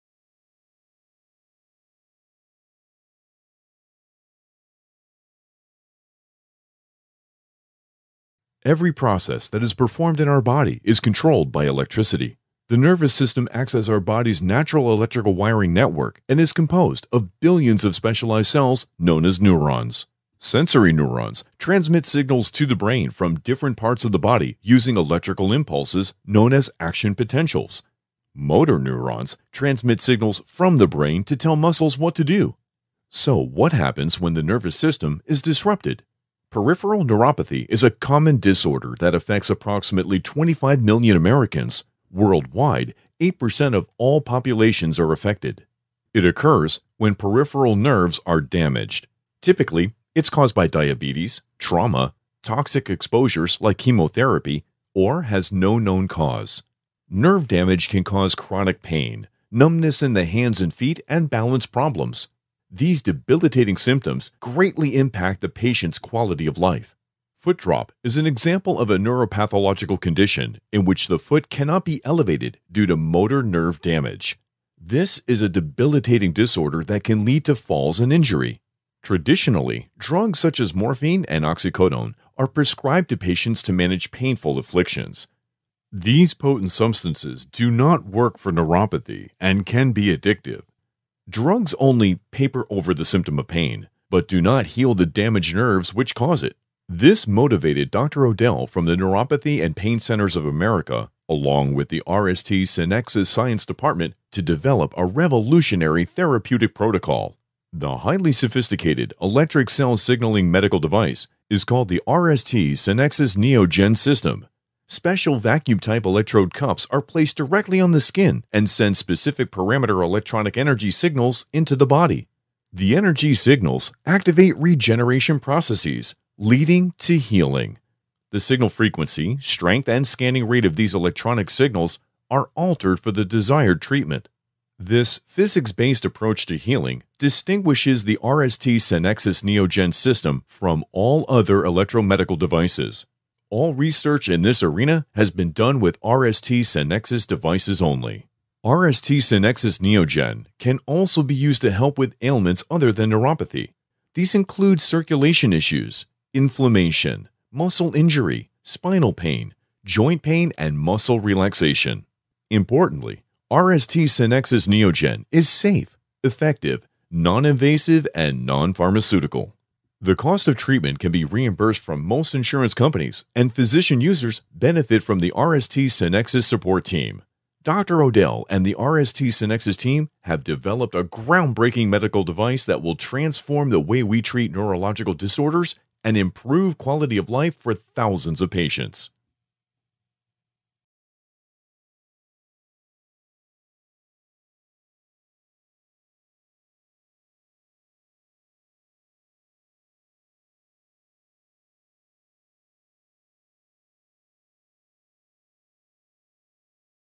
Neuropathy and other difficult to treat pain conditions
RST SANEXAS  A revolutionary treatment for neuropathy and other difficult to treat pain conditions
Dr. Odell from the 'Neuropathy and Pain Centers of America’ along with the RST-Sanexas science department have developed a revolutionary therapeutic protocol for neuropathy. The RST-Sanexas neoGEN system uses electrical cell signalling to activate regeneration processes and promote healing.